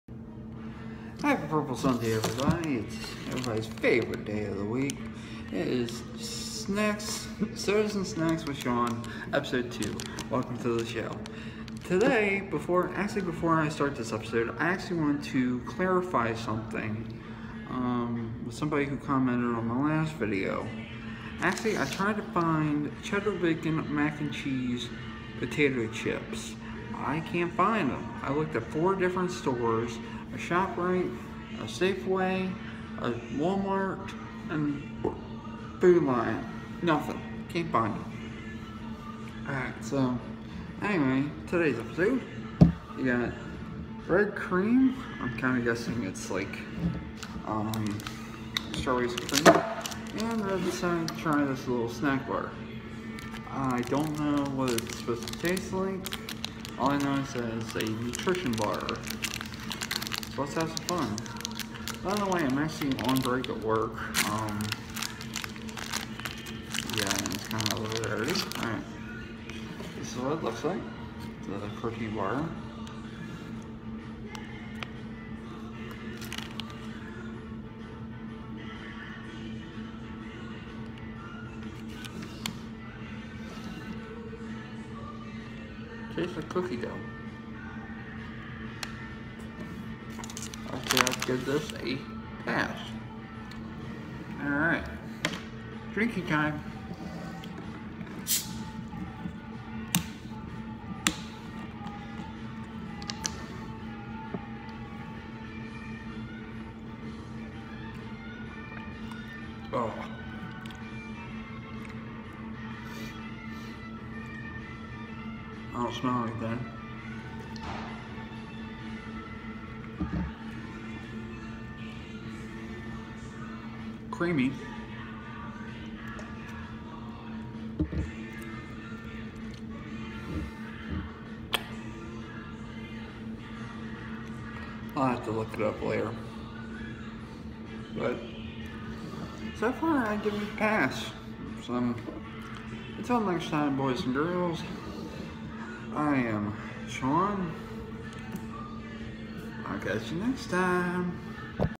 Soda & Snack with Sean
Episode 2: Kutztown Red Cream Soda & Balance Cookie Dough bar.